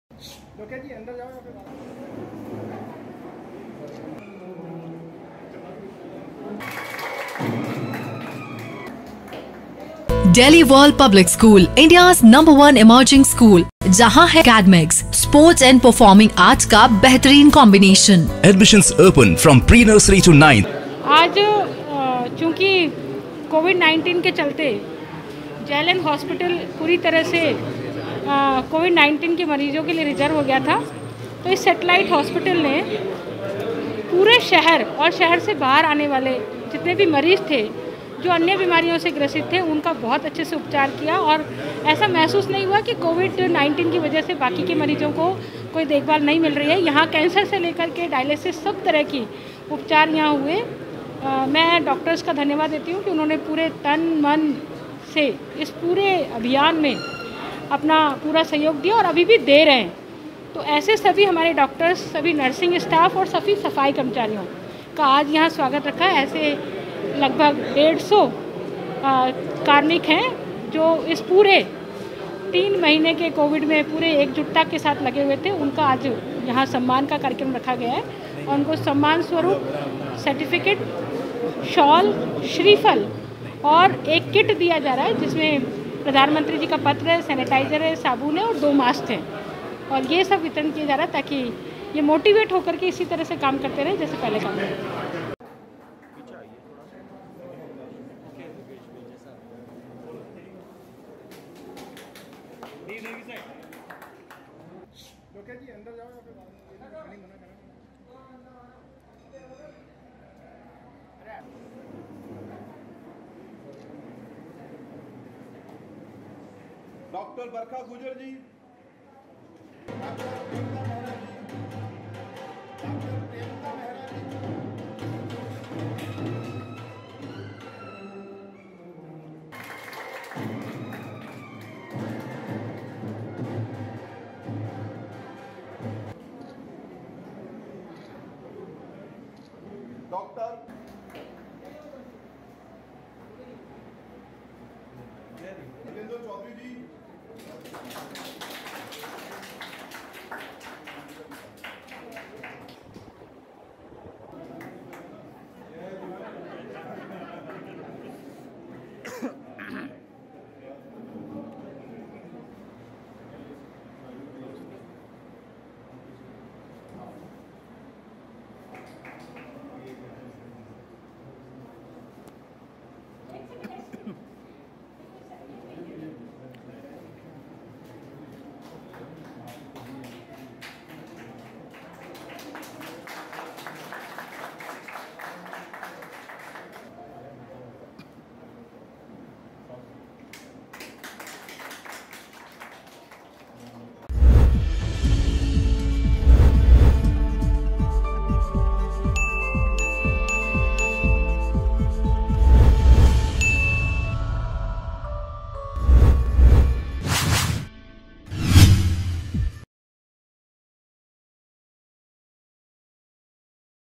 Ajmer | आदर्श नगर स्थित सेटेलाइट हॉस्पिटल में कोरोना योद्धाओं का सम्मान कार्यक्रम | MTTV INDIA
आदर्श नगर स्थित सेटेलाइट हॉस्पिटल में कोरोना योद्धाओं का सम्मान कार्यक्रम
कार्यक्रम में विधायक अनिता भदेल और बीजेपी शहर अध्यक्ष पीएस हाडा रहे मौजूद
करीब 190 योद्धाओं को सैनिटाइजर किट, सर्टिफिकेट, शॉल देकर किया सम्मानित
लॉक डाउन के समय सेटेलाइट के चिकित्साकर्मियों ने शहरवासियों को दी चिकित्सा सेवाएं